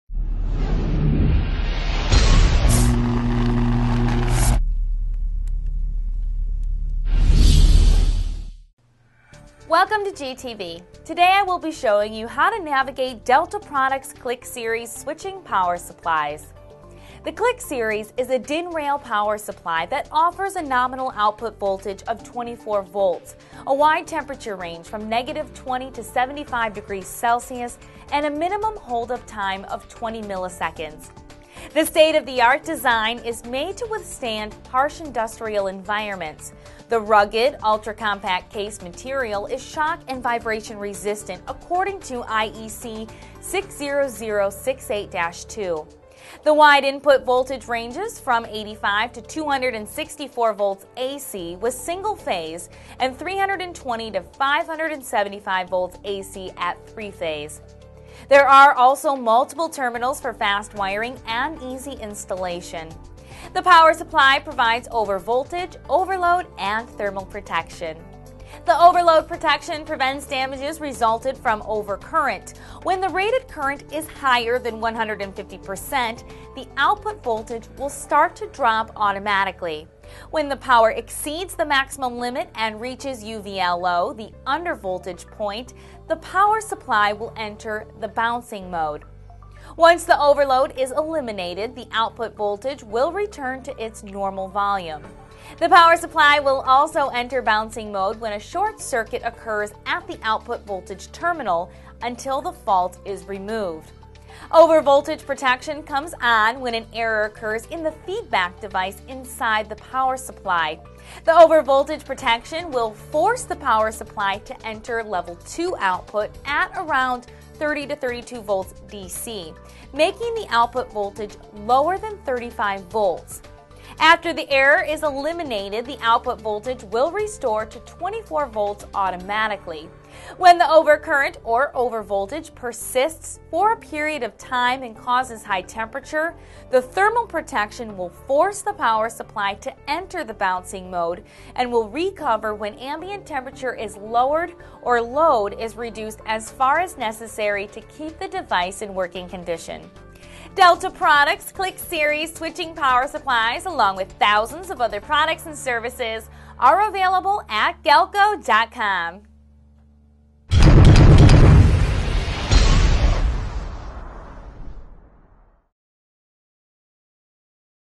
Delta Products CliQ Series Switching Power Supply Overview
Delta Products CliQ Series Switching Power Supply presented by Katie Rydzewski for Galco TV.

CliQ Series Features:
-    Easy wiring
-    Overload protection
-    Thermal protection
-    Expected life time: 10 years
-    Compact design for easy handling
-    Overvoltage protection
-    RoHS Compliant
-    Redundancy: Yes (with external O-ring diode)
-    Power boost 150% for 3 seconds

VIDEO TRANSCRIPT (partial):
Welcome to G-TV!
Today, I will be showing you to navigate Delta Products Cliq Series Switching Power Supplies.
The Cliq series is a DIN rail power supply that offers a nominal output voltage of 24 Volts, a wide temperature range from -20° to 75 ° Celsius and a minimum holdup time of 20 milliseconds. The state-of-the-art design is made to withstand harsh industrial environments. The rigged, ultra-compact case material is shock and vibration resistant according to IEC 60068-2. The wide input voltage ranges from 85 to 264 Volts AC with single phase and 320 to 575 Volts AC at 3 phase.  There are also multiple terminals for fast wiring and easy installation. The power supply provides overvoltage, overload and thermal protection.
Delta Products Cliq Series Switching Power Supplies, along with thousands of other products and services, are available at galco.com.